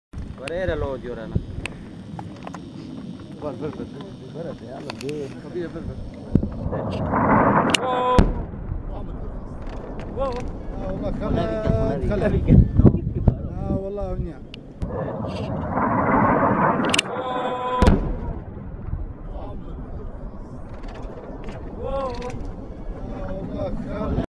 Iraq War 2015 - US Close Air Support Levels IS Position Near To Kurdish Fighters In Sinjar
Kurdish fighters filmed an US close air support air strike on a IS position in Sinjar Iraq. The Jdam levels the position located near to the kurdish fighters taking out the militants. Clashes in the area of Sinajr are actually ongoing with Kurdish Peshmergas having the upper hand thanks to the US air support.
This footage is part of an ongoing documentary of the war in Iraq through raw combat footage.